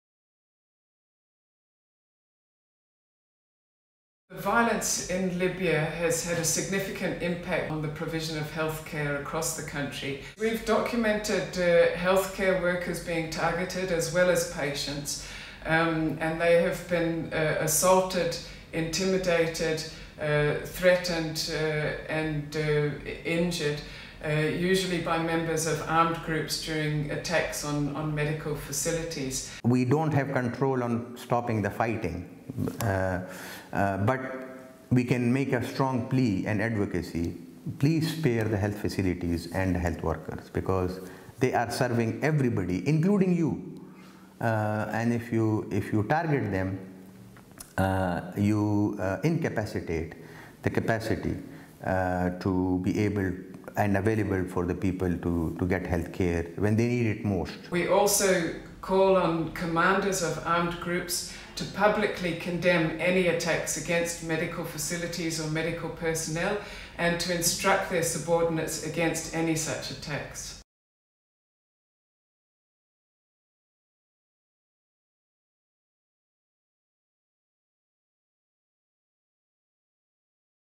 Healthcare under attack in Libya
The ongoing violence in Libya is crumbling the health care system – direct targeting of health workers and patients in the conflict and this could constitute war crimes, a UN joint report has said.

The UN Support Mission in Libya (UNSMIL) and the UN Human Rights Office (Office of the High Commissioner for Human Rights – OHCHR) in a report have documented the “devastating impact” of the violence on the delivery of health-care.